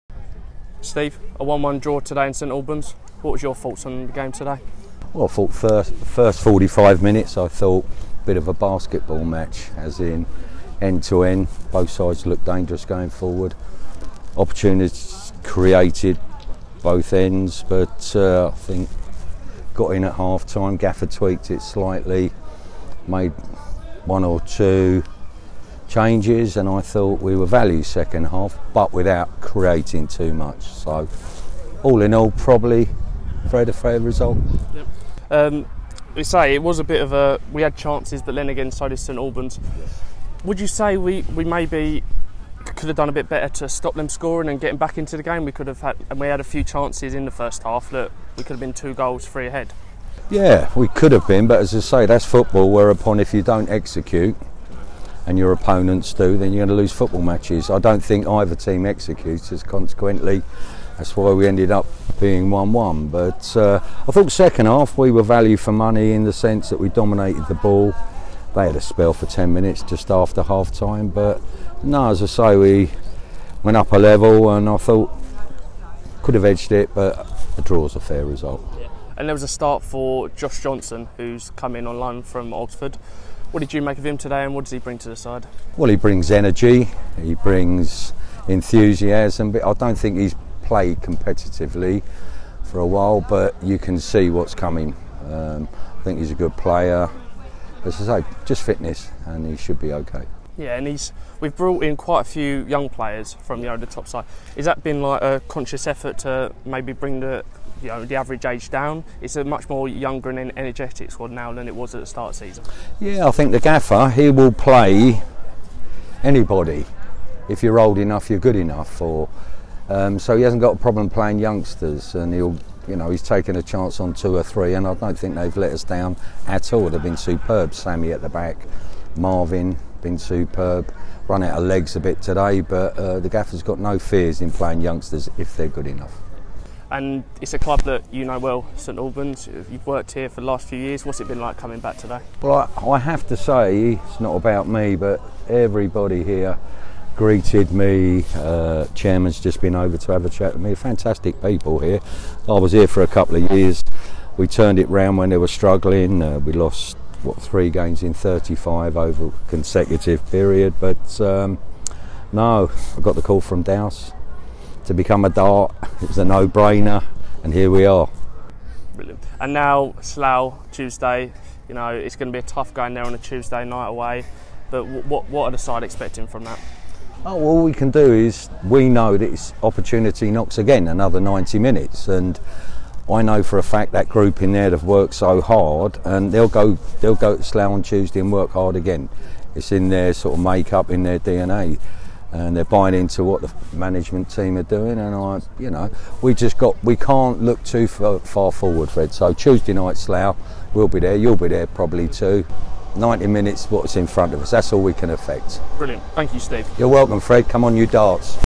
Steve Haughney | Post St Albans City (A)
Hear from coach Steve Haughney following a 1-1 draw away at St Albans City.